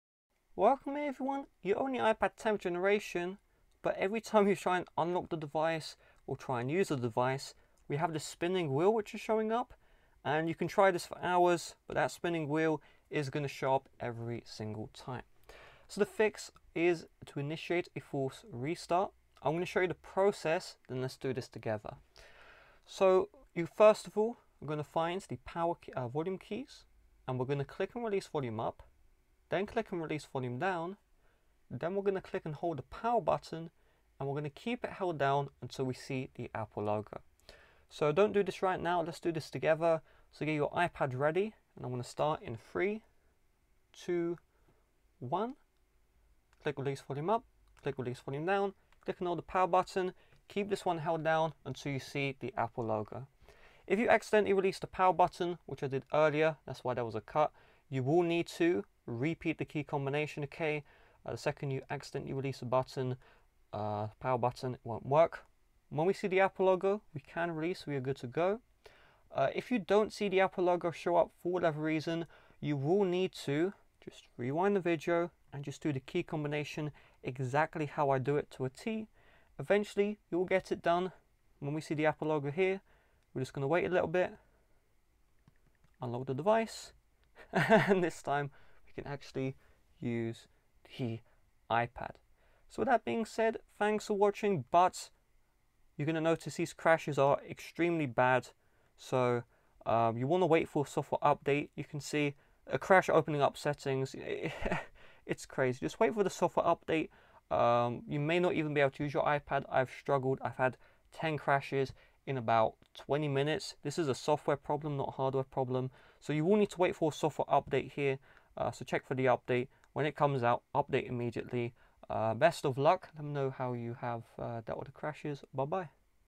iPad 10th Generation Spinning Wheel Black Screen SOLVED!
Learn how to fix spinning wheel black screen on iPad 10th Generation. This should also work on other iPad models as well.